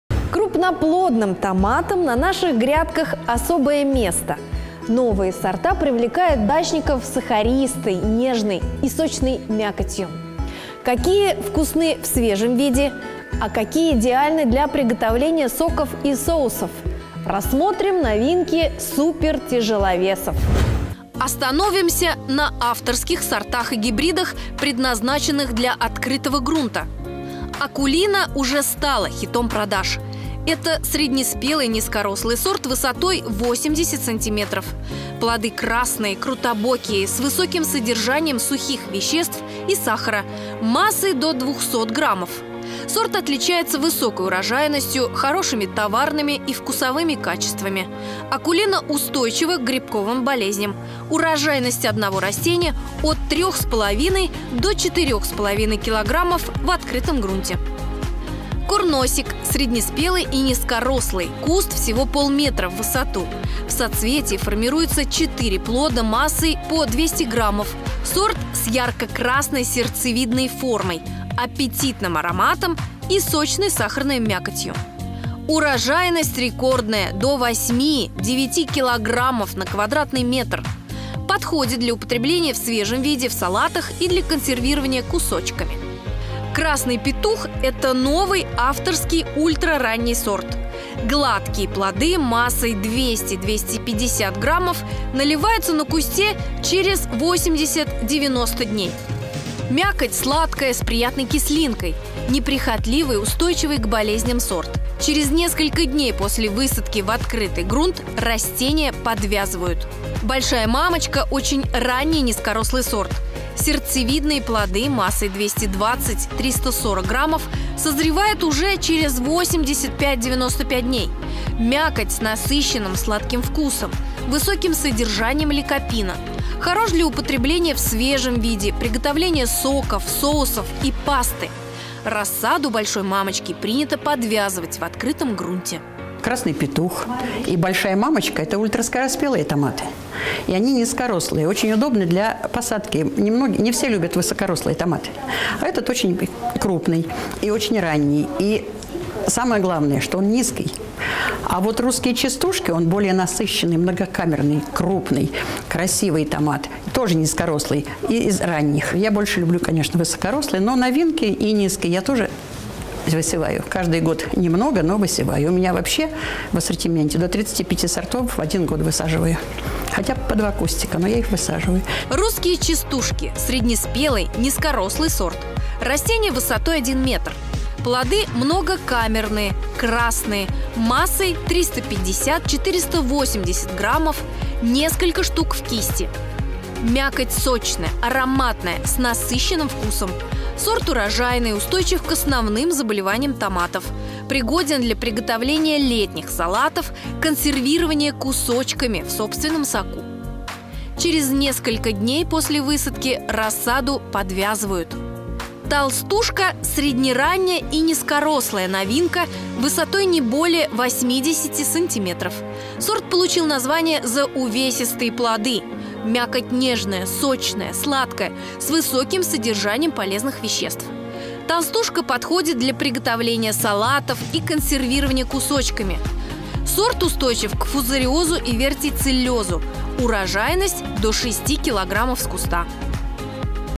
Удачные заметки. Крупноплодные томаты
Крупноплодным томатам на наших грядках - особое место. Новые сорта привлекают огородников сахаристой, нежной и сочной мякотью. Какие вкусны в свежем виде, а какие подойдут для консервирования соков и соусов? Рассмотрим новинки  супертяжеловесов.